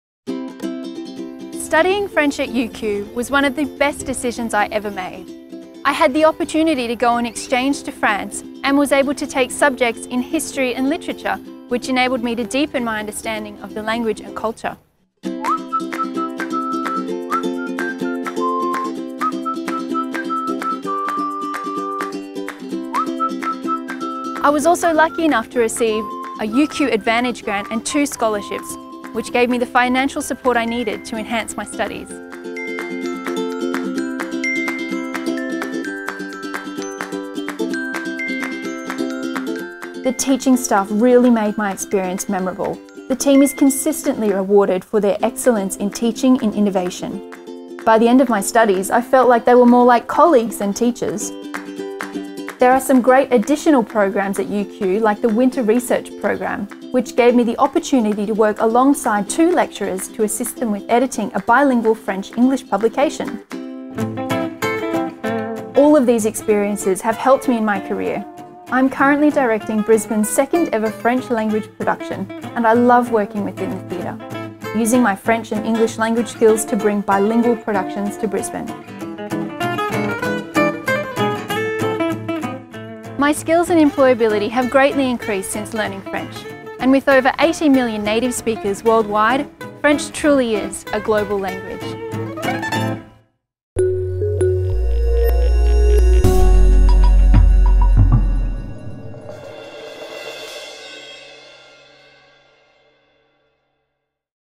Studying French at UQ
What can you expect studying French as part of your UQ Bachelor of Arts program? Emily Miller shares her experiences of studying in the School of Languages and Cultures and the opportunities she had at UQ. CRICOS Code 00025B